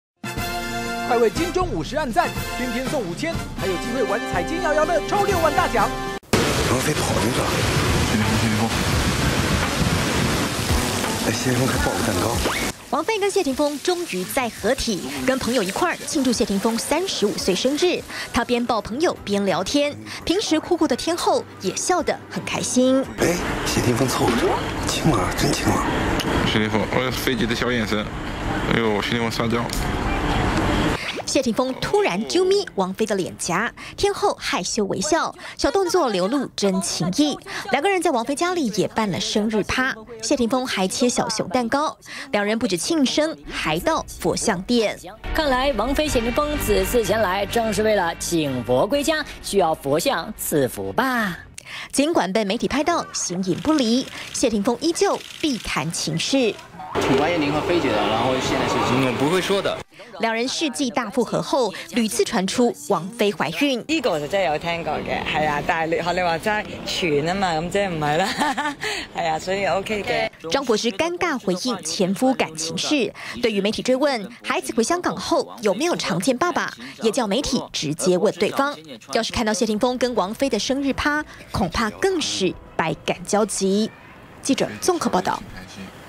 20150923中天新聞 謝霆鋒慶生啾咪王菲 求佛賜福放閃
謝霆鋒跟王菲世紀大復合後，對戀情相當低調，不過，日前兩人一起慶祝男方35歲生日，謝霆鋒還當眾啾咪王菲臉頰，天后也嬌羞微笑。小動作流露濃濃情意。